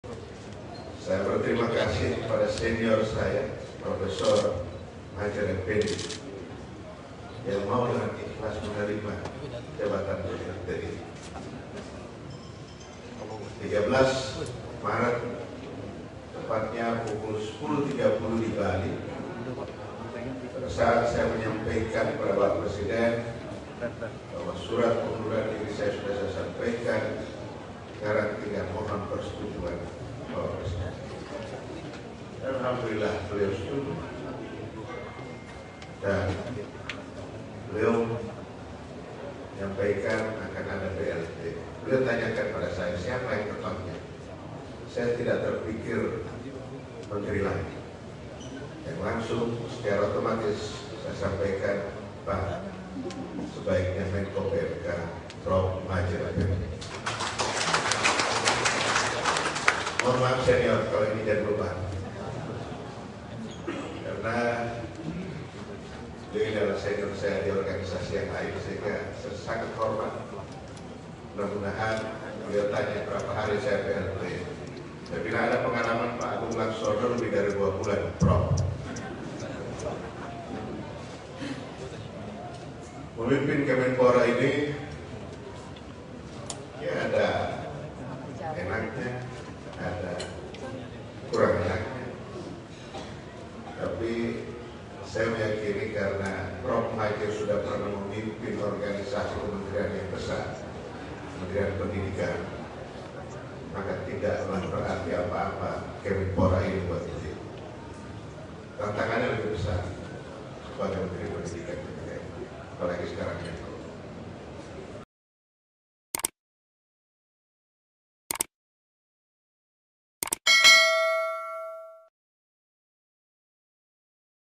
Cerita Zainudin Amali Ditanya Presiden Sosok yang Layak Jadi Plt Menpora
KOMPAS.com - Wakil Ketua Umum PSSI Zainudin Amali menceritakan momen dirinya menghadap Presiden Jokowi untuk menyerahkan surat pengunduran diri sebagai Menteri Pemuda dan Olahraga.

Saat itu, ia mengaku ditanya langsung oleh Jokowi terkait sosok yang layak menjabat sementara sebagai plt Menpora.

Zainudin mengatakan ia langsung menjawab Menko PMK Muhadjir Effendy. Menurutnya, Effendy layak karena merupakan senior Zainudin.

Video Jurnalis: Nissi Elizabeth
Penulis Naskah: Nissi Elizabeth
Video Editor: Nissi Elizabeth
Produser: Okky Mahdi Yasser